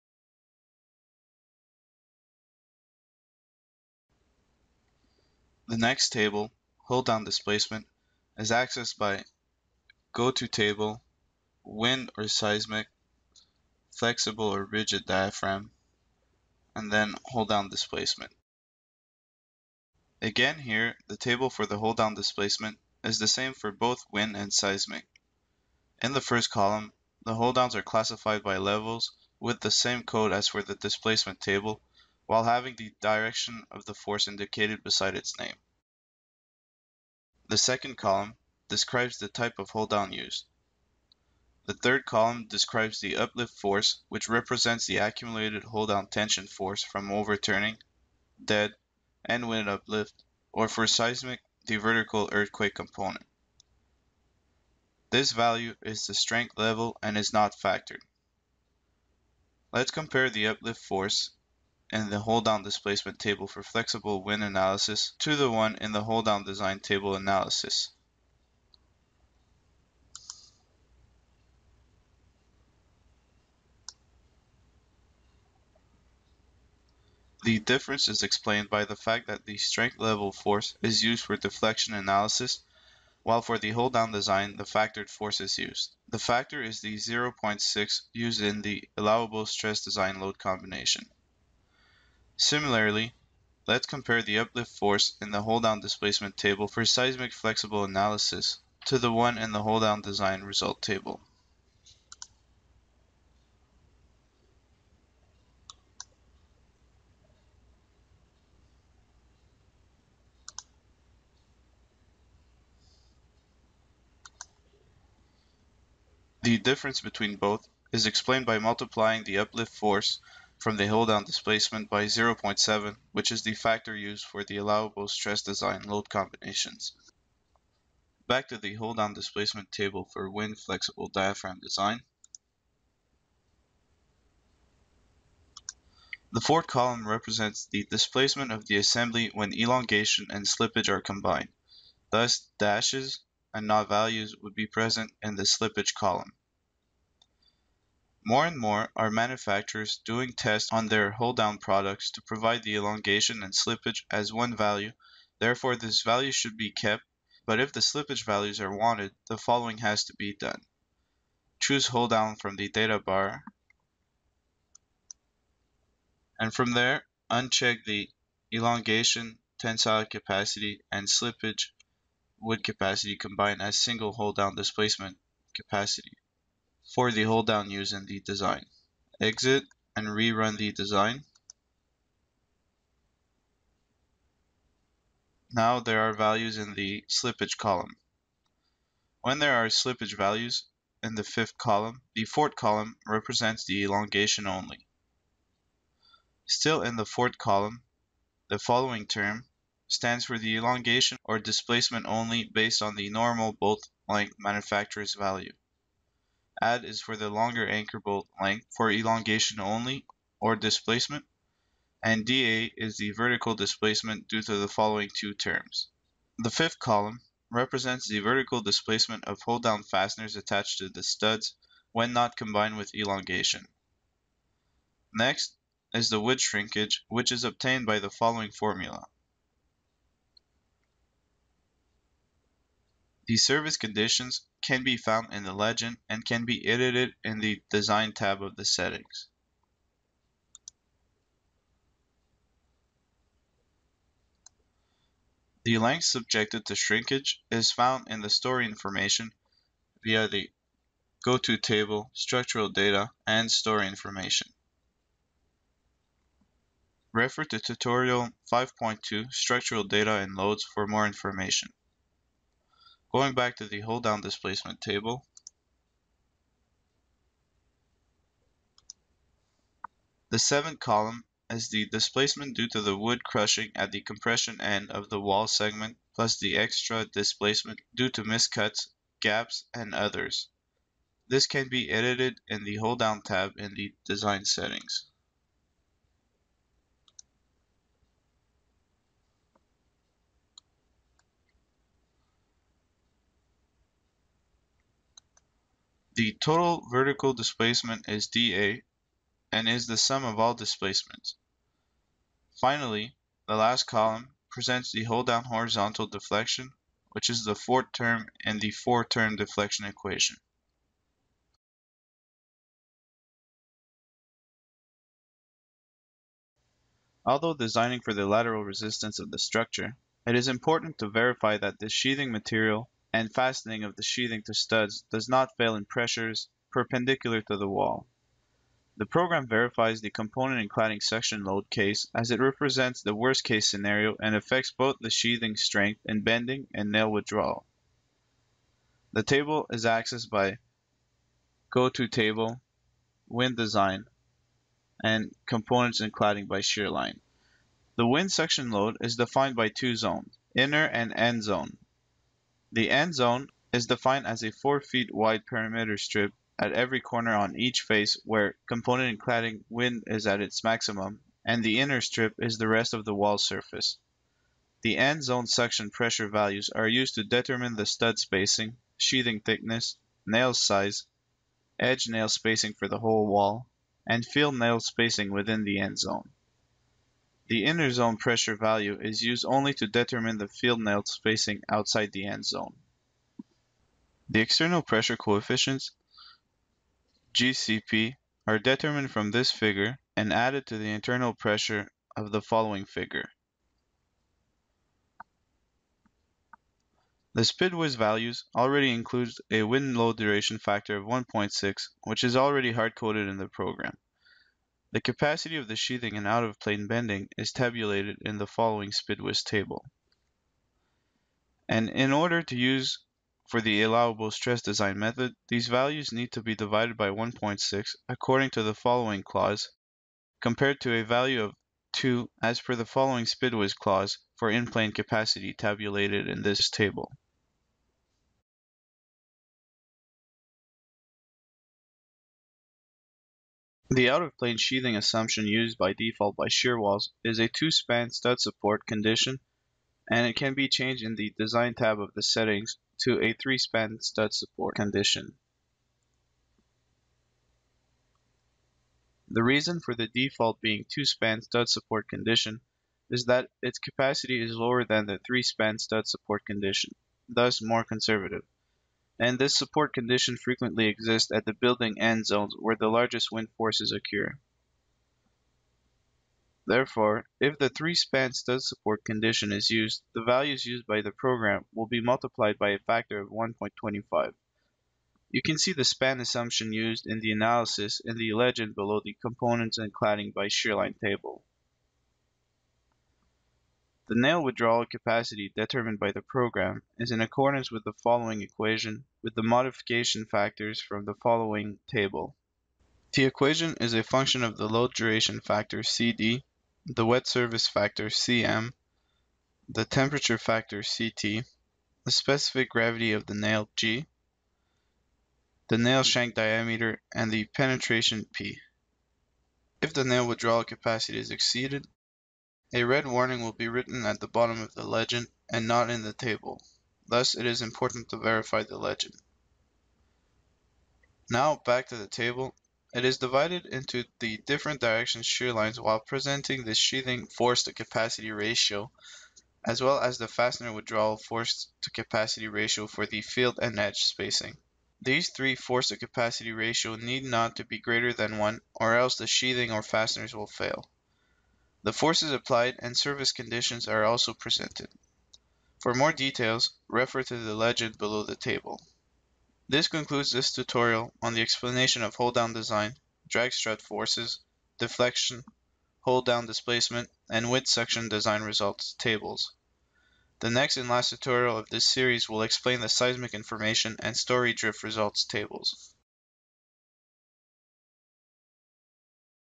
Shearwalls Tutorial 5.4-B - Deflection, Hold-downs, Drag Strut and Wind Suction
Shearwalls Tutorial 5.4-B explains how to interpret the deflection, hold-downs, drag strut and wind suction results.